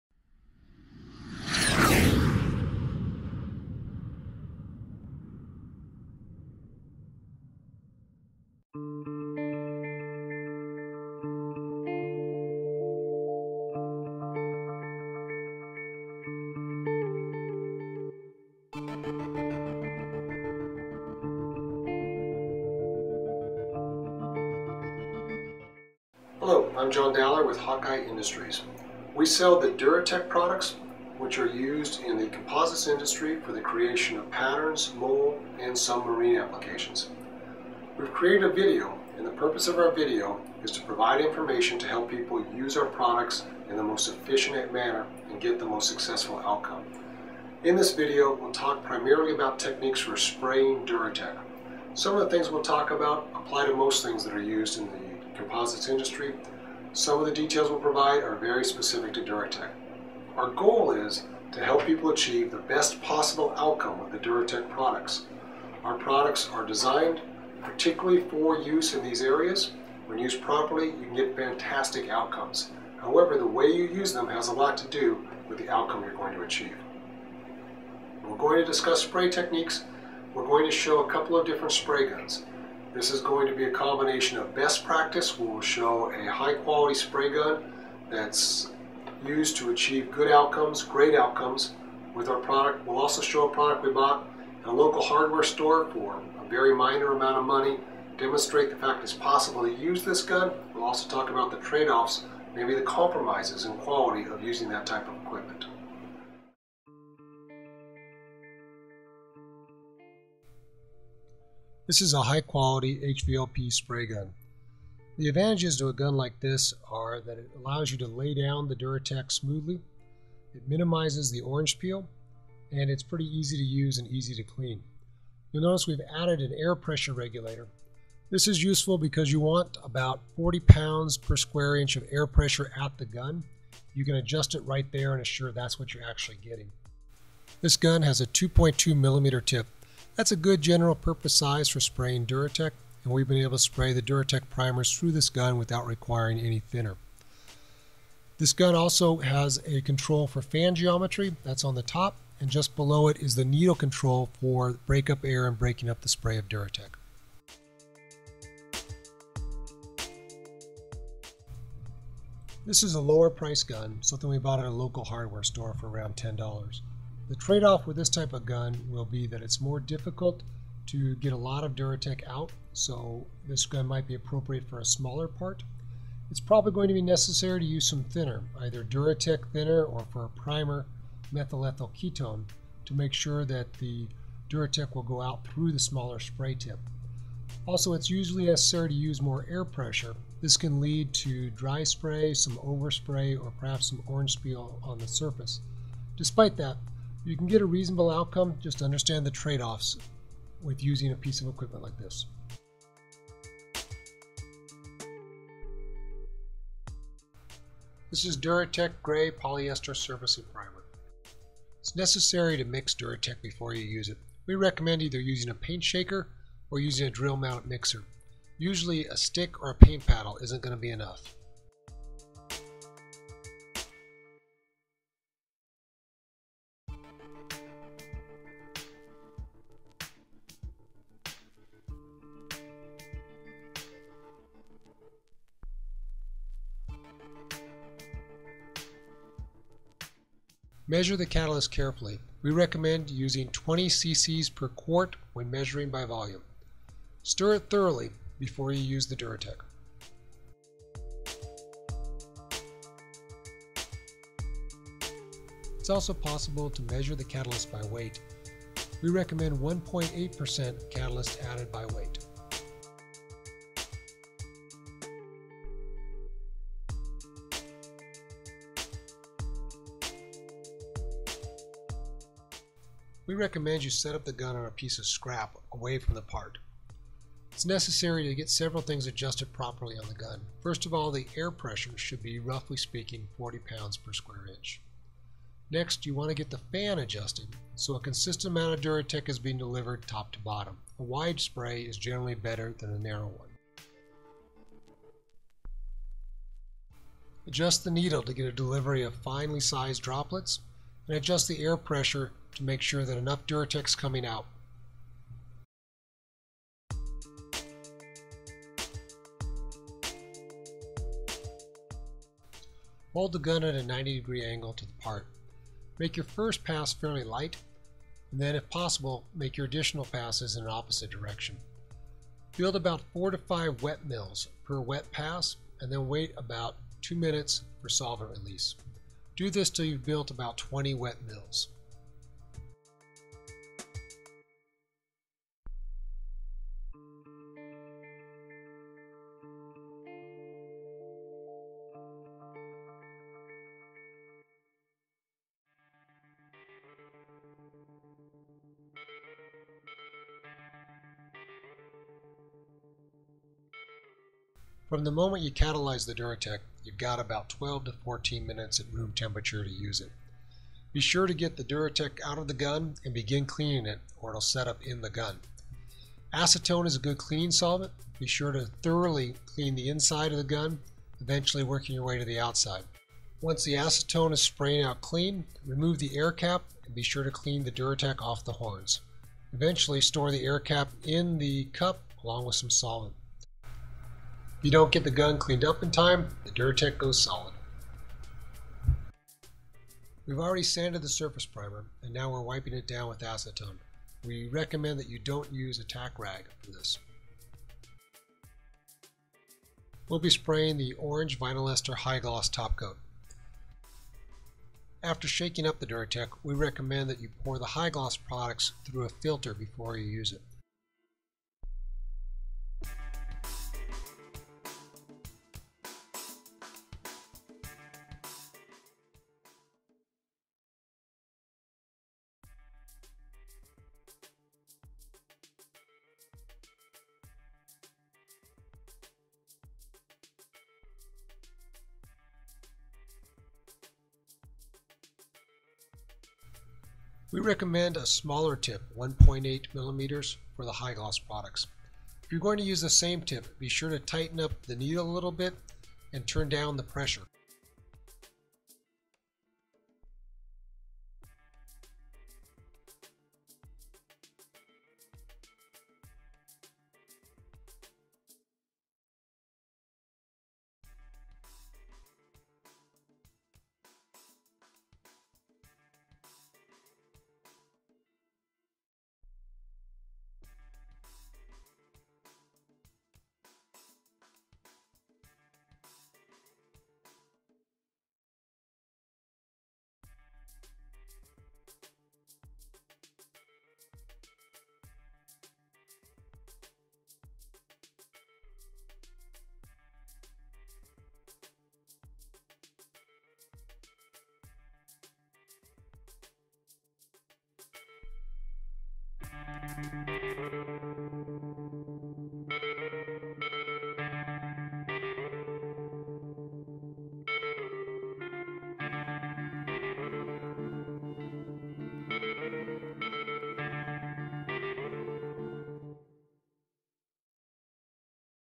How to Spray Duratec
Getting the right performance from our Duratec primers and topcoats is achieved by better understanding application.  Follow along in this video with the Hawkeye Industries team for application guidance, including general spray gun details, selecting tip size, proper catalyzing, and spray techniques.

Looking for more application details on the Duratec product you're working with?  Check out the product Technical Data Sheets found on our website, which include application and troubleshooting guides.